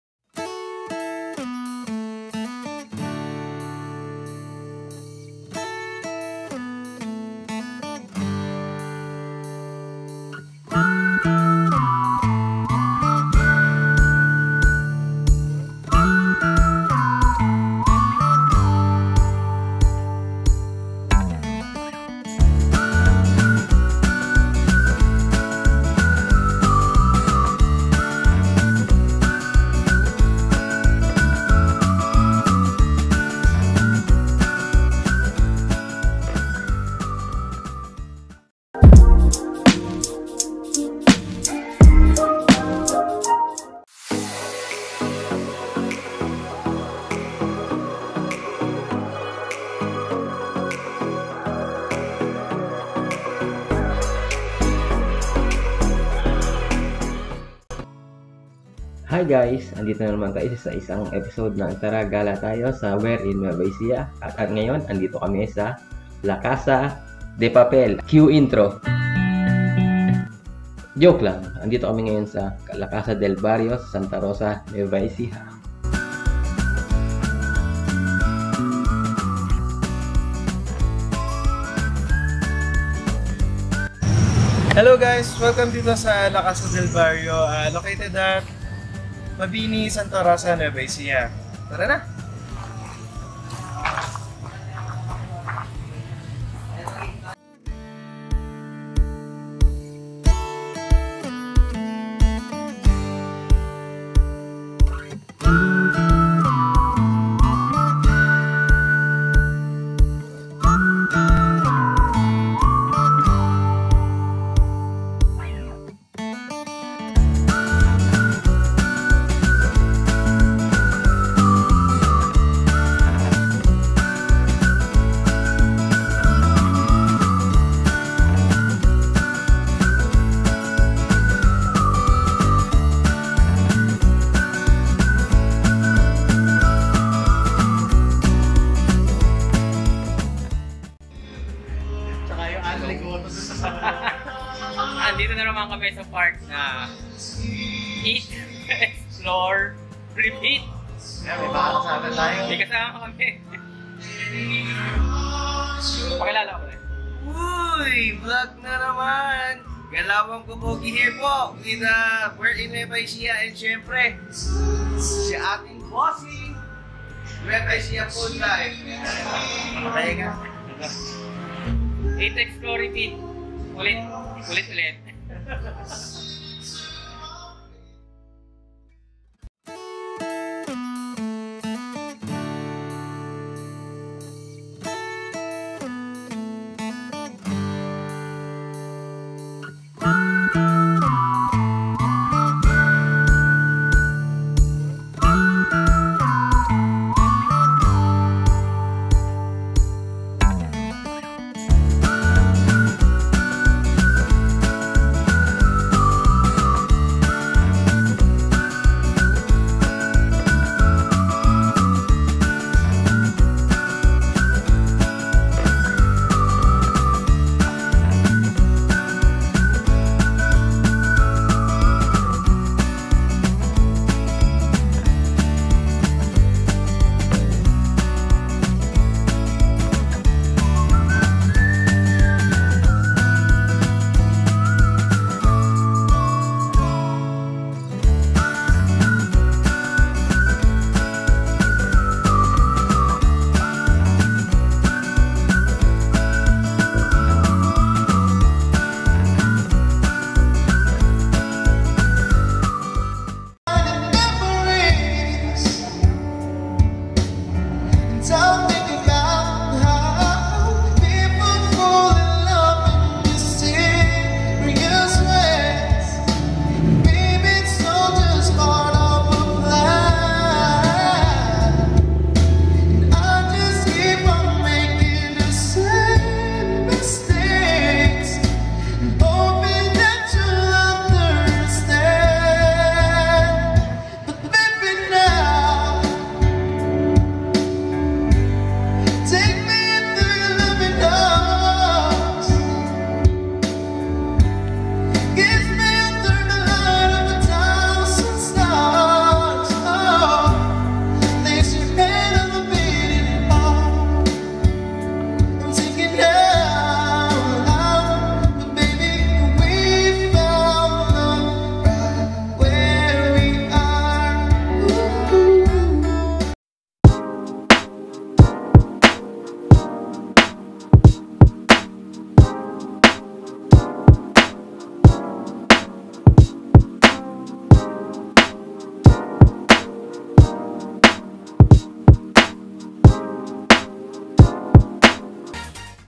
Lumang Bahay na Ginawang Restaurant - La Casa Del Barrio
The house was built in 1890. Renovated in 1974 and finished in 1977. The great descendants of Capt Gregorio Del Barrio (founder of San Gregorio) have initiated renovating in this era and converted into a restaurant that provides the finest cuisines to taste.

Del Barrio El Restoran
Located at Brgy Mabini Santa Rosa Nueva Ecija